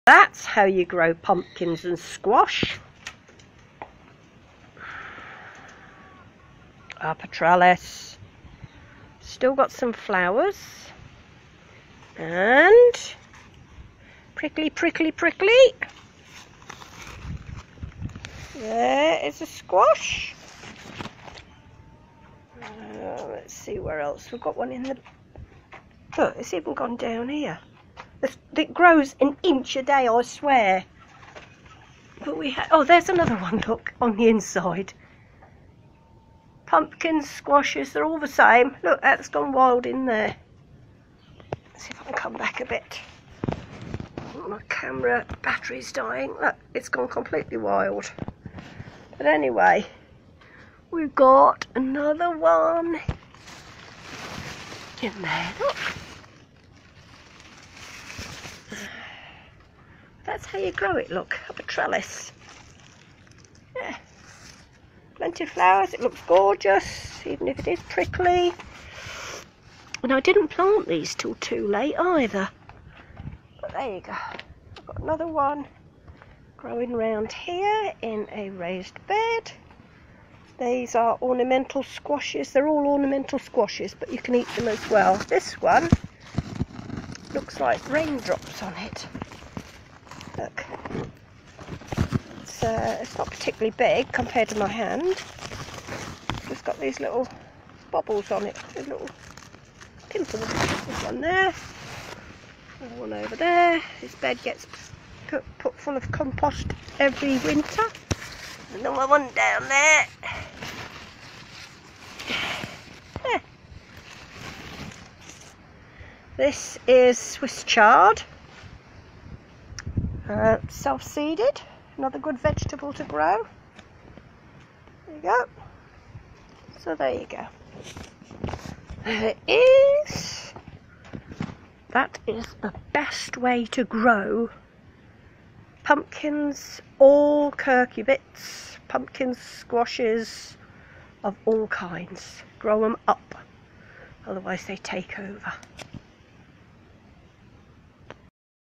squashes
the best way to grow squash, cucumber, courgettes,if space is at a premium.